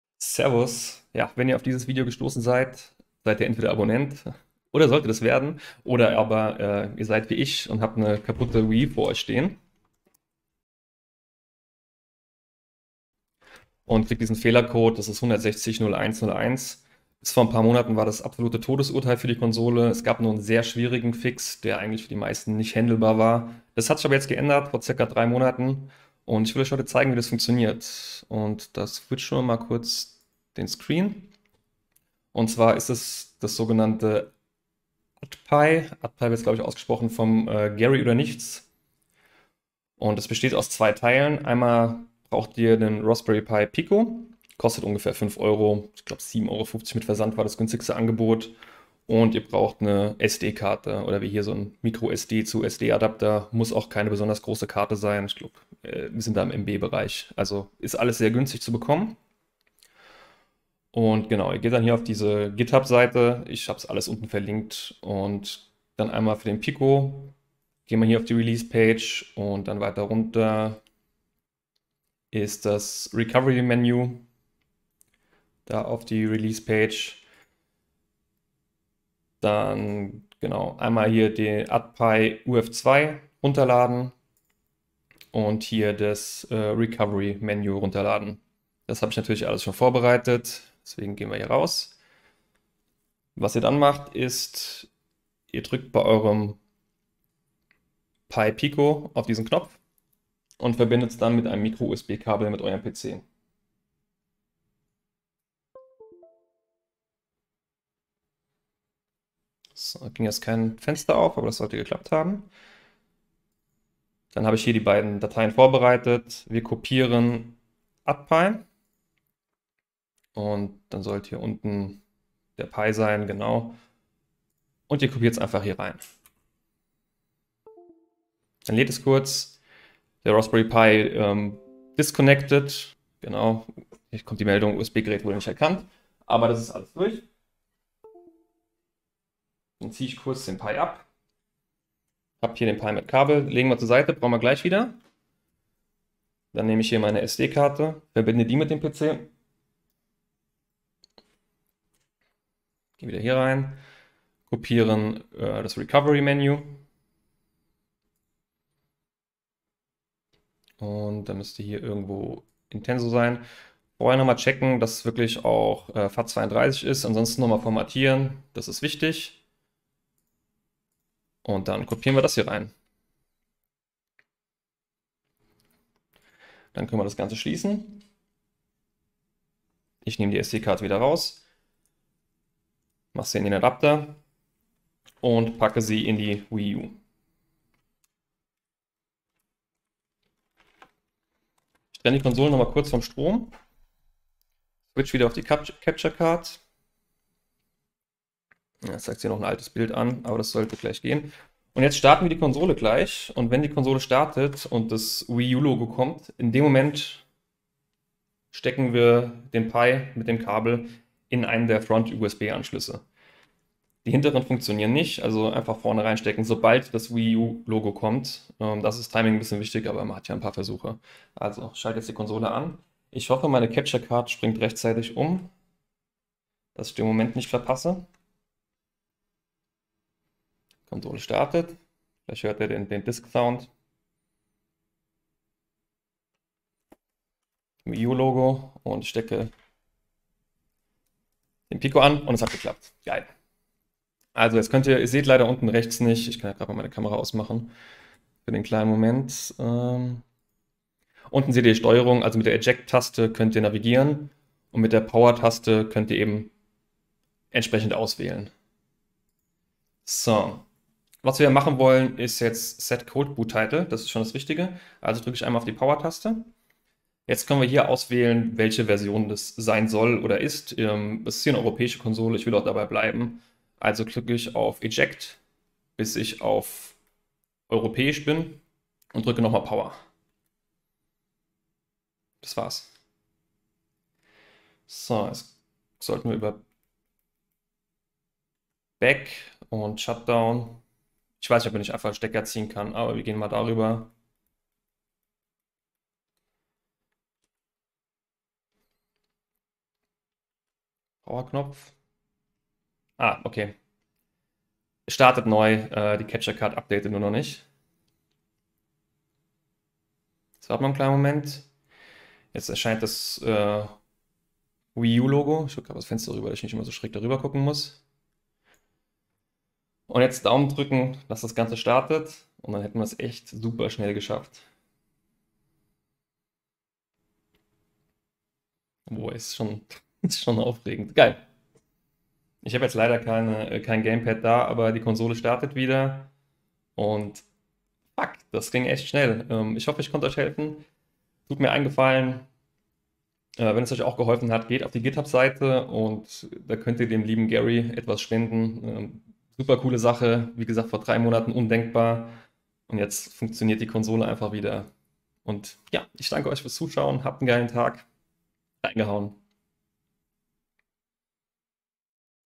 Wii U UNBRICK - Reparatur mit UDPIH (German)
In diesem Video erkläre ich euch wie Ihr eine gebrickte Wii U (z.B. Error 160-0101 / CBHC brick) mit UDPIH repariereren könnt. Dieser Fehler tritt zum Beispiel auf wenn man eine gemoddete Konsole (Homebrew / CBHC) zurücksetzt.

Um die Wii U zu unbricken benötigt Ihr einen Raspberry Pi Pico sowie eine SD-Karte.

Raspberry Pi Pico
microSD mit Adapter